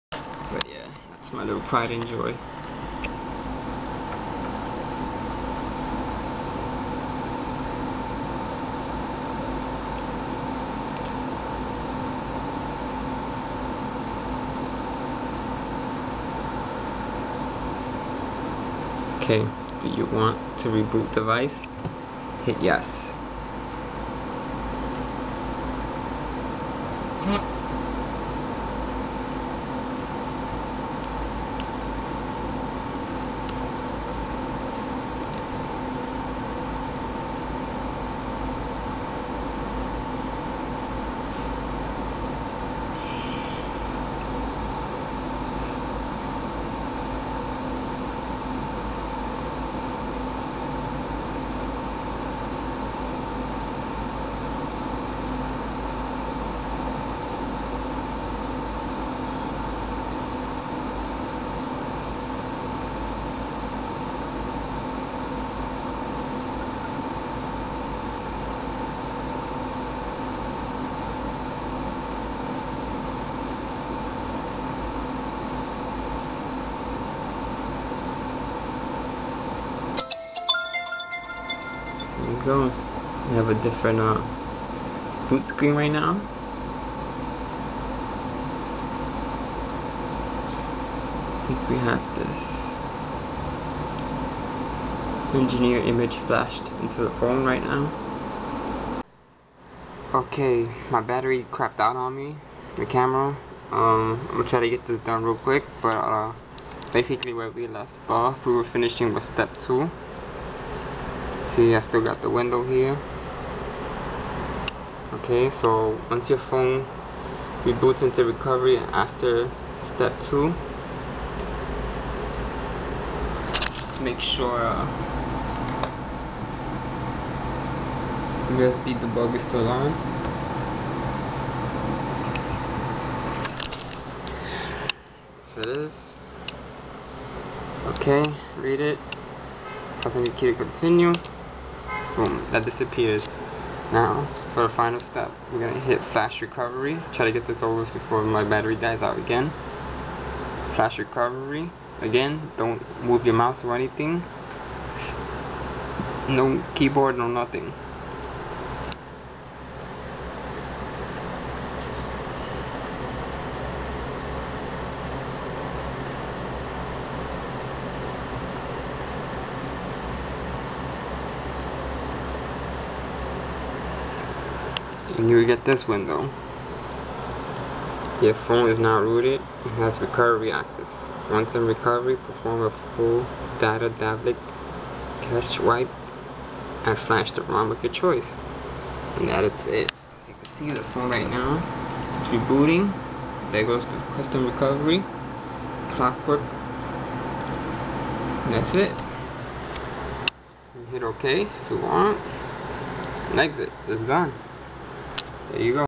Simple Root .1 a 3 Step root for HTC Evo 4G Part 2-2 + Part 3
Simple Root .1 3 Step root for HTC Evo 4G

Part 2 of 2 + Part 3

The battery on my camera died out on me while recording which explains the obvious jump in the video...